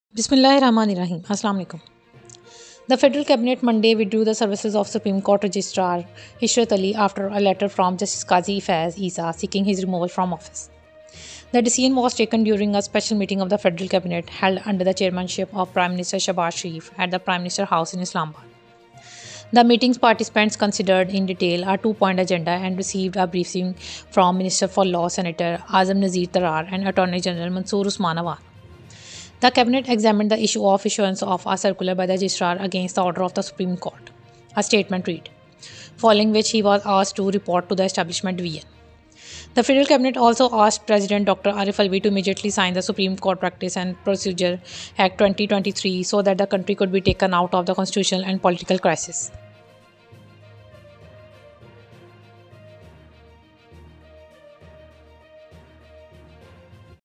Cabinet withdraws services of SC registrar after Justice Isa's letter
ISLAMABAD: The federal cabinet Monday withdrew the services of Supreme Court Registrar Ishrat Ali after a letter from Justice Qazi Faez Isa seeking his removal from office.

The decision was taken during a special meeting of the federal cabinet — held under the chairmanship of Prime Minister Shehbaz Sharif — at the Prime Minister House in Islamabad.

The meeting's participants considered, in detail, a two-point agenda and received a briefing from Minister for Law Senator Azam Nazeer Tarar and Attorney General Mansoor Usman Awan.

The cabinet examined the issue of issuance of a circular by the registrar against the order of the Supreme Court, a statement read, following which he was asked to report to the Establishment Division.